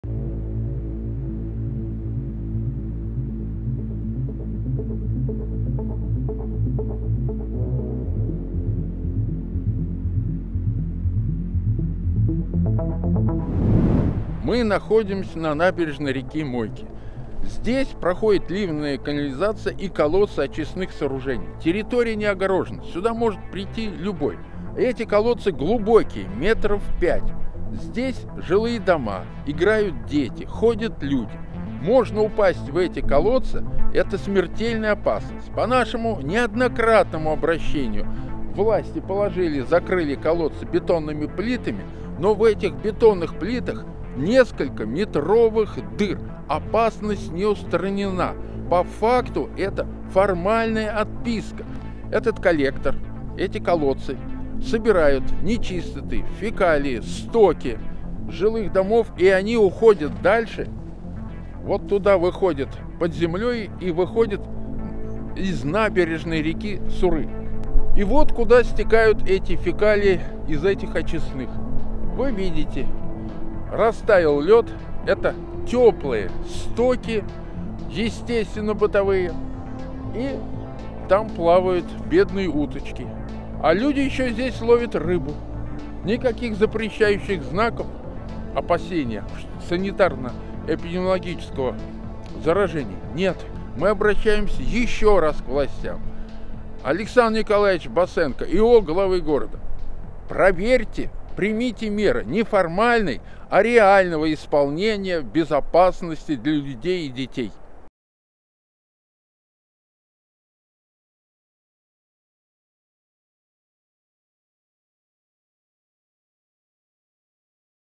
Три канализационных коллектора могут стать смертельной ловушкой для жителей Пензы
В этом видео речь пойдет о смертельной ловушке в виде открытых канализационных коллекторов, глубиной несколько метров, расположенных в непосредственной близости от жилых домов.

Жители Пензы неоднократно сообщали чиновникам из администрации Пензы о проблеме, писали и просили помощи у депутатов, но вместо решения проблемы получали банальные отписки.

Как можно видеть, в нескольких метрах от многоэтажек, расположен опасный объект, три канализационных коллектора. Территория не огорожена, никем не охраняется и может стать смертельной ловушкой для любопытной детворы. Об этом известно городским чиновникам, однако мер никаких не предпринимается.

Отсутствие должного внимание к объекту повышенной опасности, представляет серьезную угрозу и может привести к трагическим последствиям.